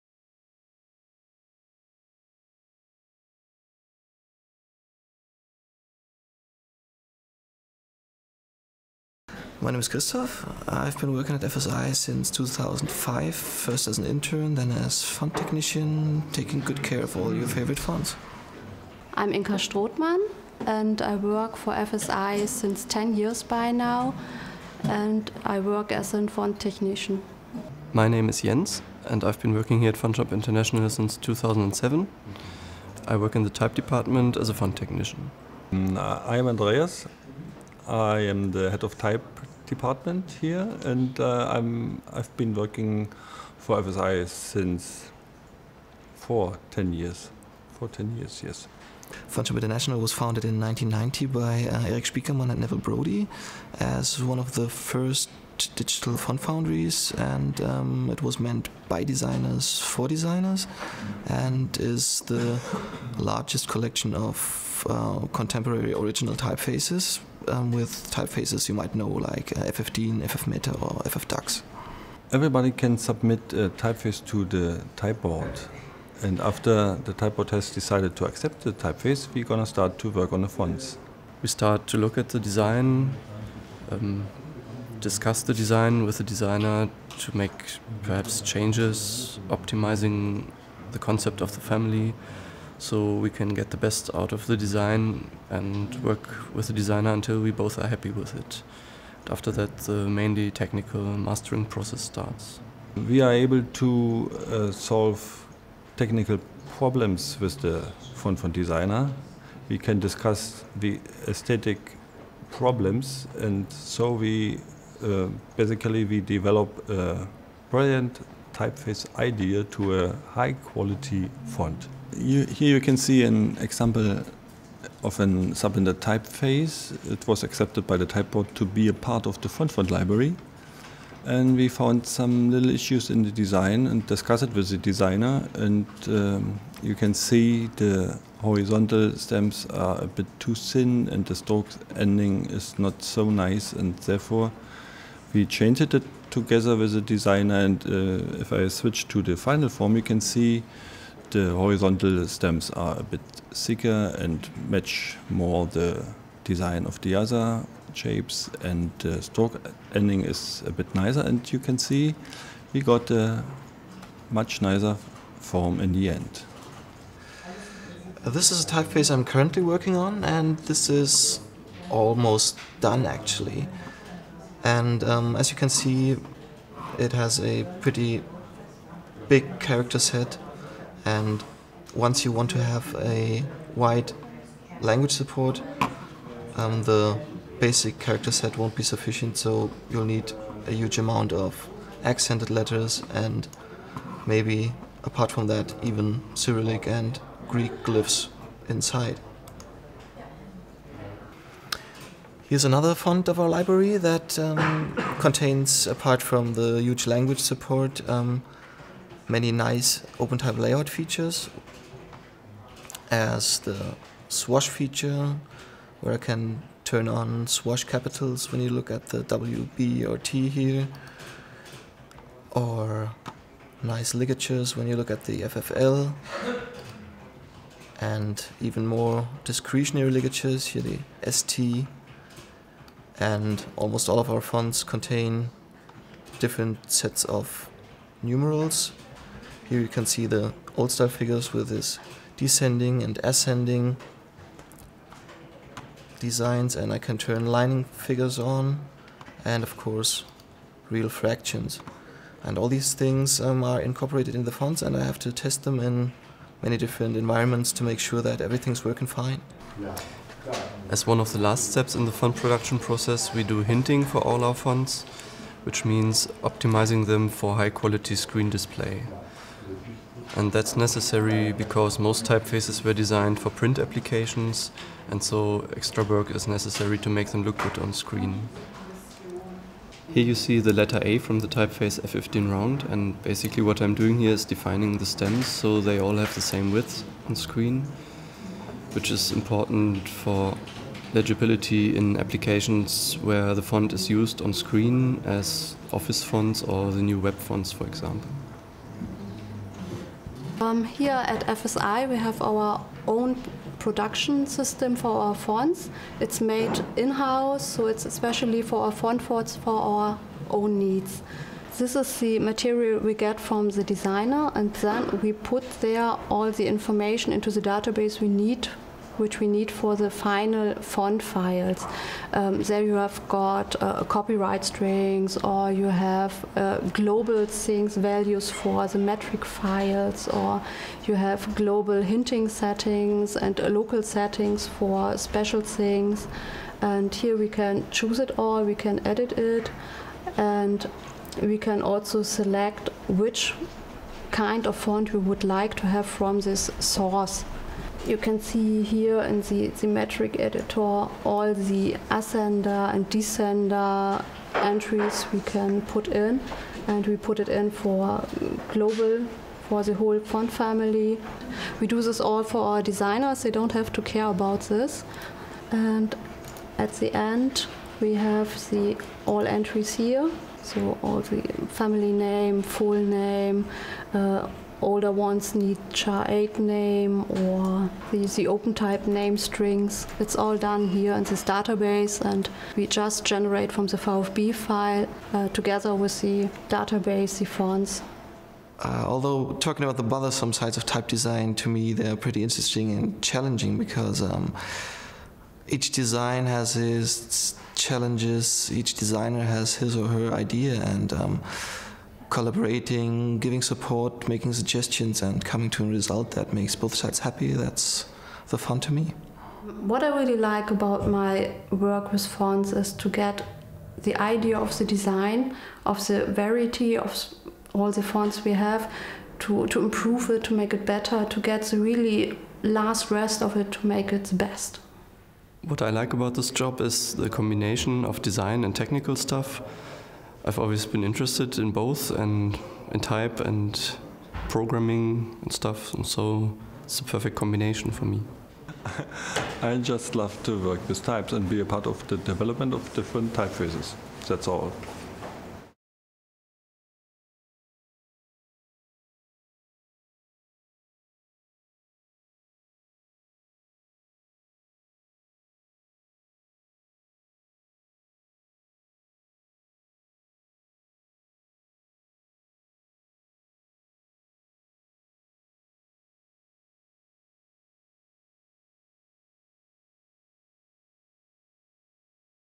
FontCast #16 — FontFont Type Department
FontFont represents some of the most talented and interesting type designers in the world. And behind every of these great type designers is FontFont's great technical team. Andreas Frohloff, head of the Type Department at FontShop International, explains: "We develop brilliant typeface ideas into high quality fonts". This rare peek behind the scenes reveals how these unsung heroes work together with the type designers, providing aesthetic feedback and technical assistance, to ensure that the resulting fonts meet FontFont's high standards.